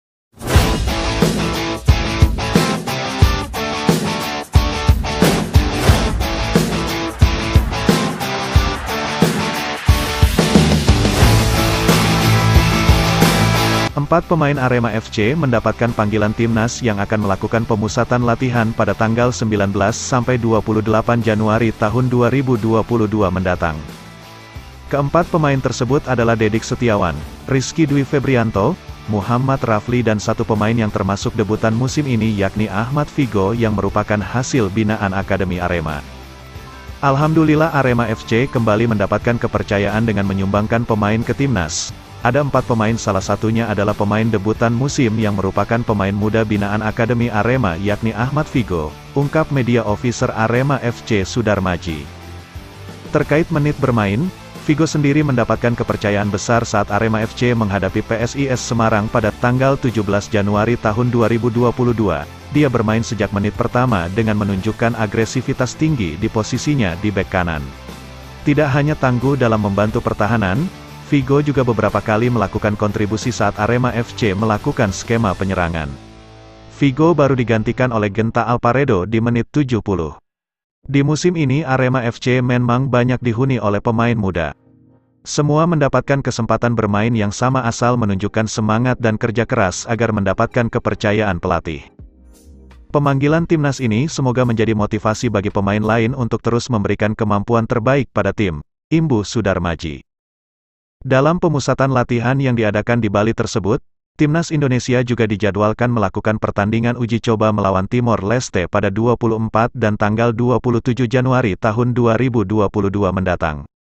Berita Arema Hari ini, 4 Pemain Singo Edan! Dipanggil Shin Tae Yong Untuk FIFA Match Day
Sumbang 4 Pemain untuk Timnas, Jebolan Akademi Arema Curi Perhatian